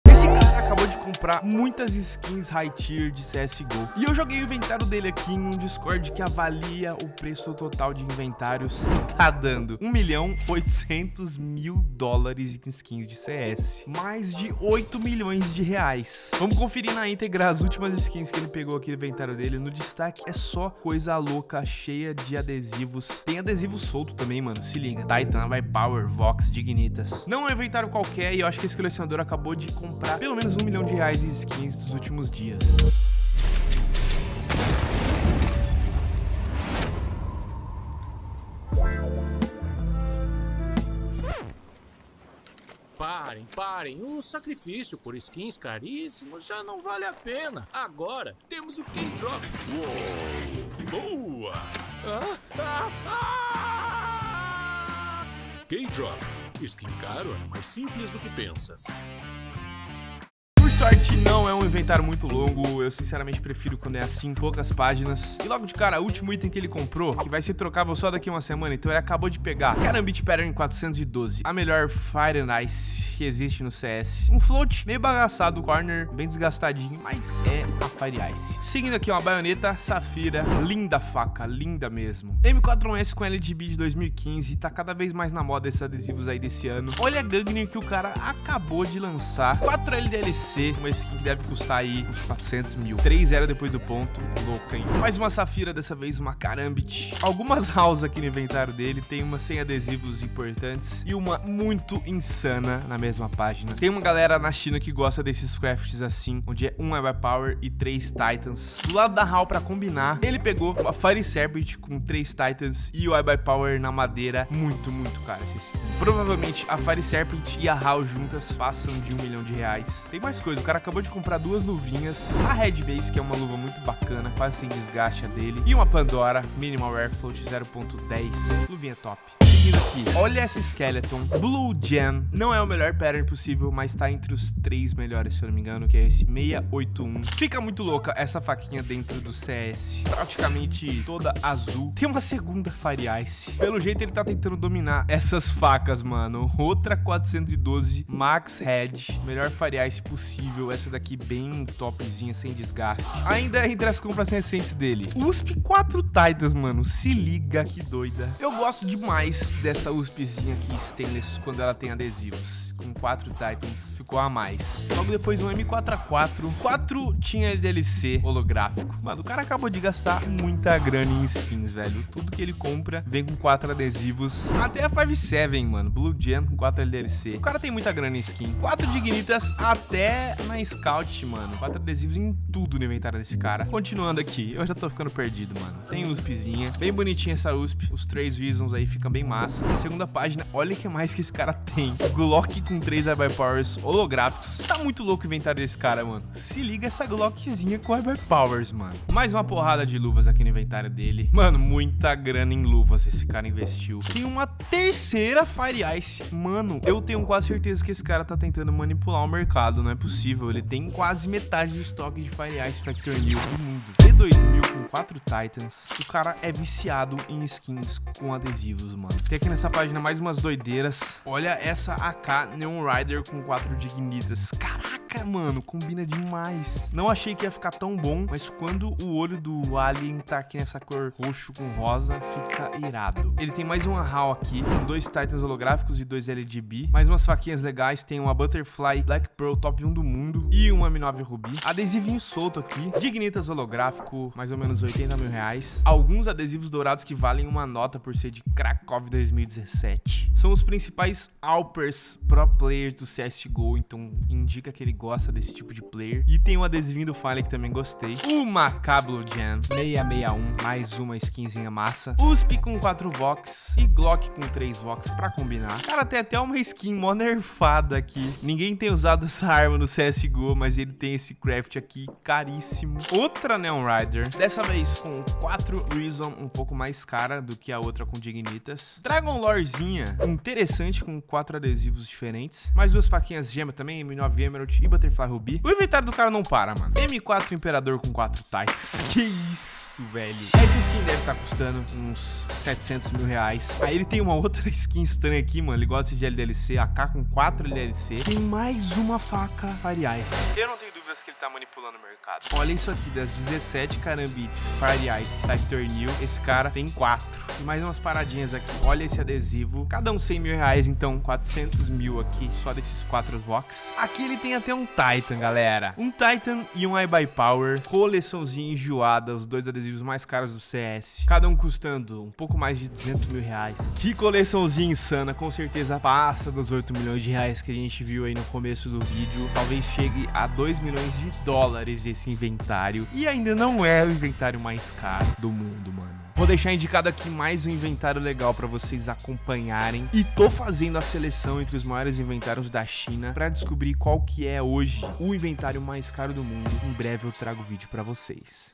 Ele Acabou De Gastar R$1.000.000 em Skins de CS:GO (inventário de R$8 milhões)
Um Colecionador de skins que tem Inventário avaliado em mais de R$8 milhões, acaba de investir mais 1 milhão de reais.